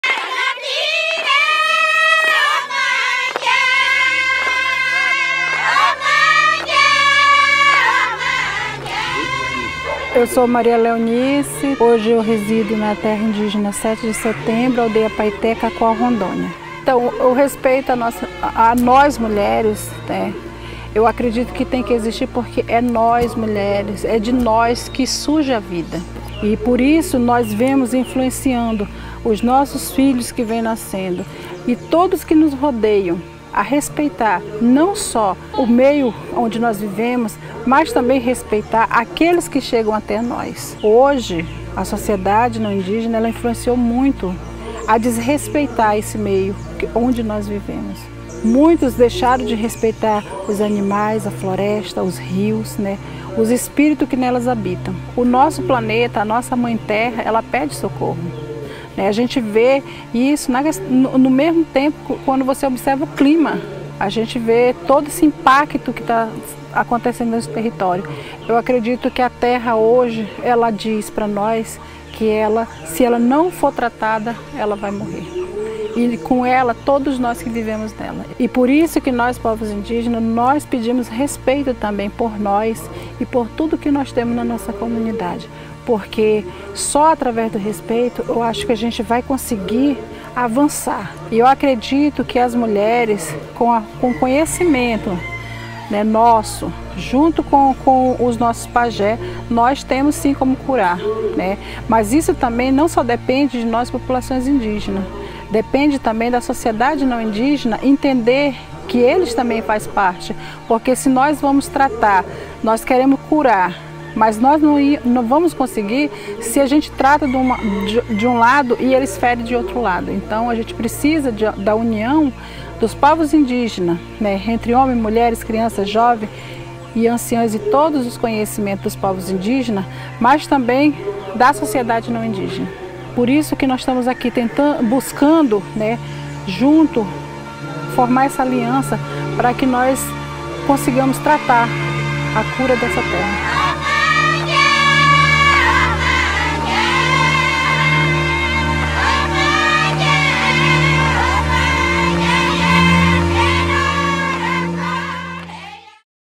CuraDaTerra - Leonice
Hoje vemos os efeitos das crises sociais, ambientais, econômicas e espirituais causadas por um modelo de crescimento infinito em um planeta finito. Nós mulheres indígenas, junto com nossas comunidades, curamos a terra, porém é tarefa de todos assumir a responsabilidade de defender o futuro.

5 de Setembro: Encontro Global de Mulheres Indígenas

CuraDaTerra EP 03 - Tsitsinã Xavante
Mídia Índia +

Este vídeo foi feito de material reciclado para gerar a menor quantidade de emissões de carbono. Com material de: Google Earth, New Atlantis, Extinction Rebellion, Nature Now, The Rules, Demarcação Já!